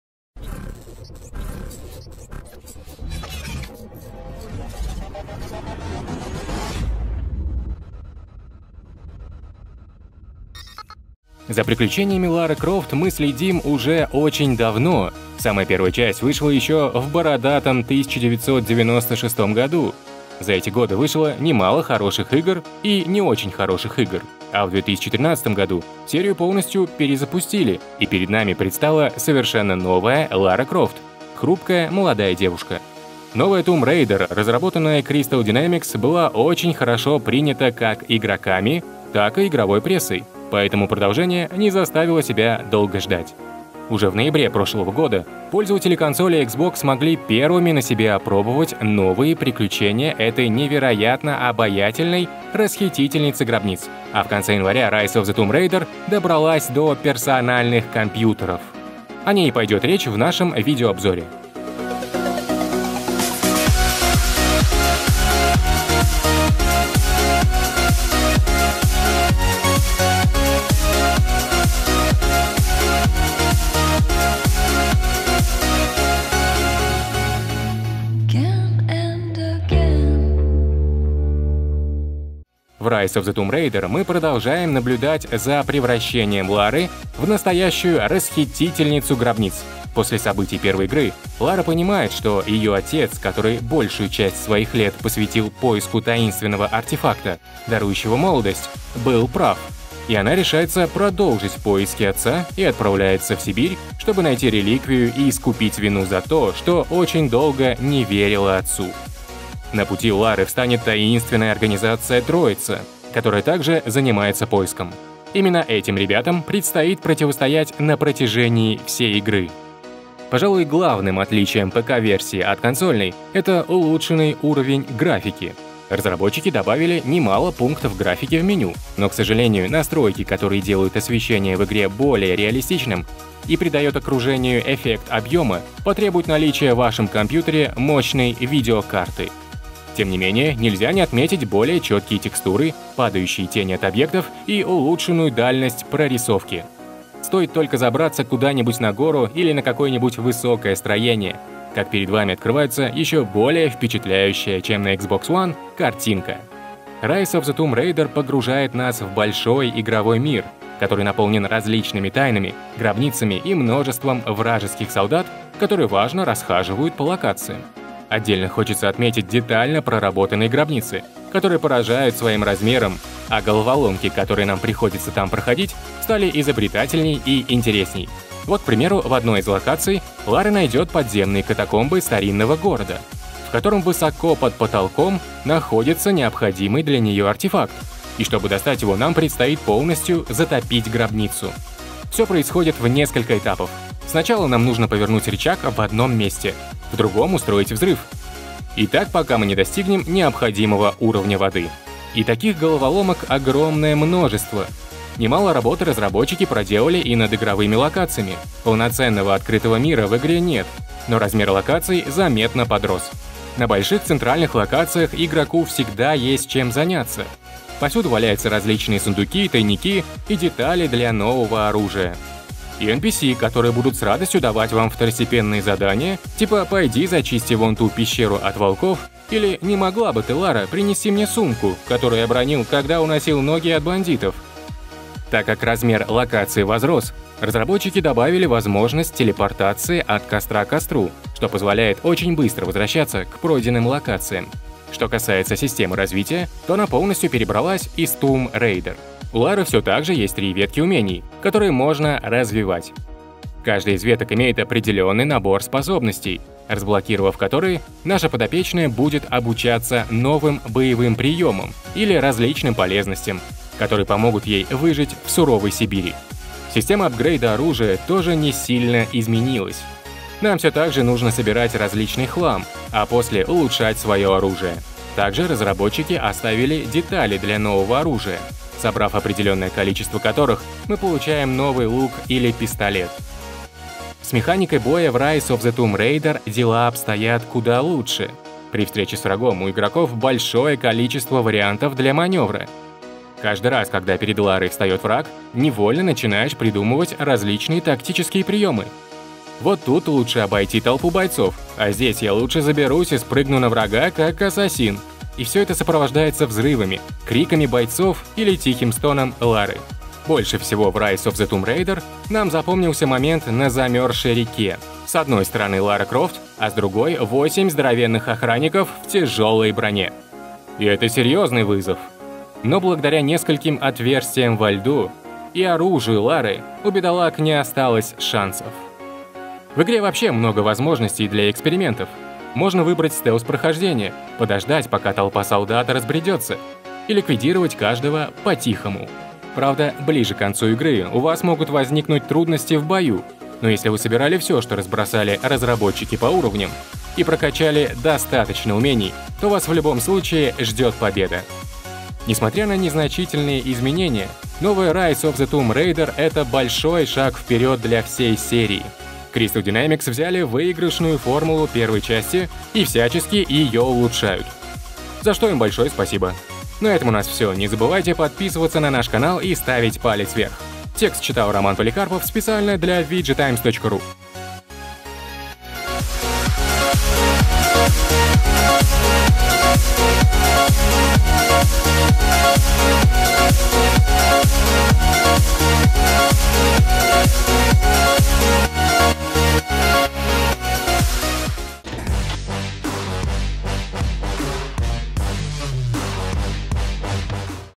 Видеообзор Rise of the Tomb Raider от VGTimes.ru
Rise of the Tomb Raider - один из главных прошлогодних эксклюзивов на консоли Xbox One, наконец-то добрался до персональных компьютеров. Что на этот раз интересного придумали разработчики из Crystal Dynamics, и чем отличается PC-версия от консольной, об этом обо всём смотрите в нашем видеообзоре.